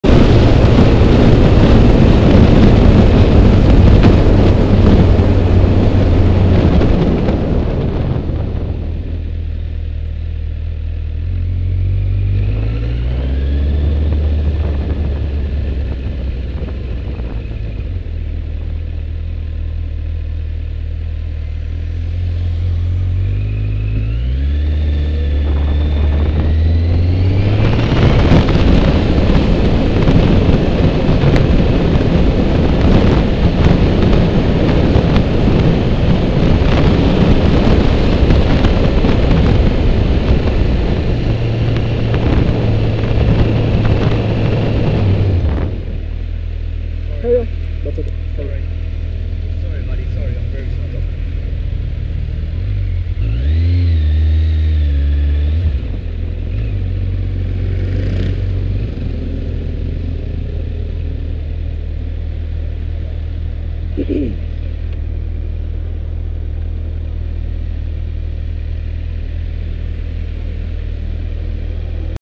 2013-05-01 Carillon ave. observations
Re-broadcast of this clip without my explicit written permission is forbidden. Read the description below.

This clip encompasses a set of observations while riding through Carillon Avenue. This area has a primary school, university, hospital and multiple clinics. It is thus fraught with plenty of potential disasters waiting to happen, so it pays to be extra-cautious.

All observations (except the last one) are non-events.